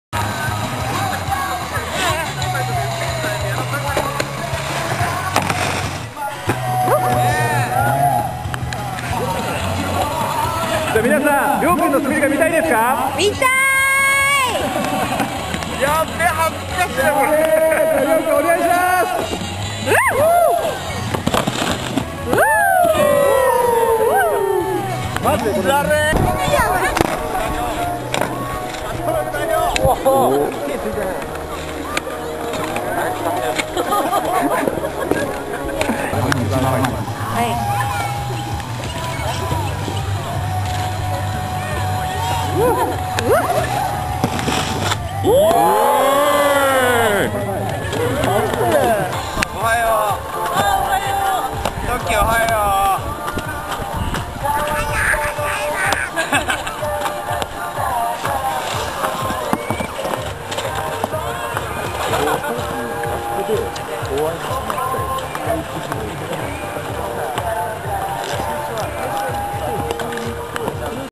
3月3日限定ムラサキスポーツプロデュースランプ
プロライダーによるデモも開催。FM新潟パーソナリティー白岩ましろさんも挑戦